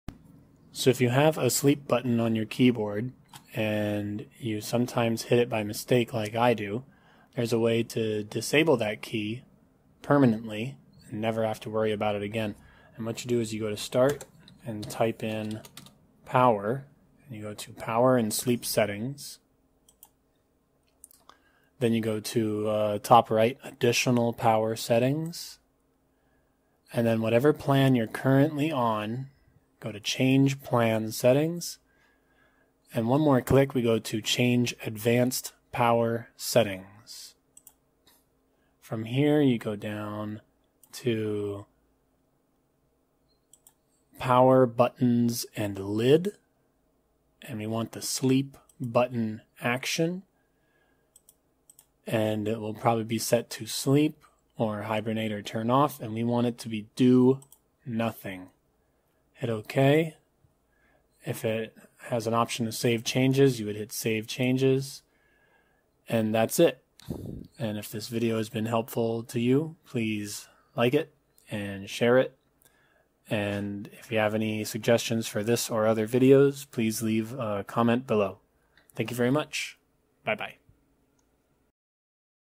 Tutorial: How to Disable your Keyboard's Pesky Sleep Button in 60 seconds with Windows
STEP 1: Press START and type "Power" and choose "Power and Sleep Settings"
Step 2: Click "Additional Power Settings"
Step 3: Click "Change Plan Settings" for your current plan
Step 4: Click "Change Advanced Power Settings"
Step 5: Click "Power Buttons and Lid"
Step 6: Click "Sleep Button Action"
Step 7: Change the option to "Do Nothing"
Step 8: Press OK to save changes.

I have a keyboard with a sleep key next to the escape key, and every so often I'll be playing a video game and accidentally hit SLEEP instead of escape. If you ever have a similar problem to me, then this will fix it for you permanently, no extra software required. The feature to disable this key is built into every Windows 10 computer. I imagine it's about the same for windows 7 and 8 as well.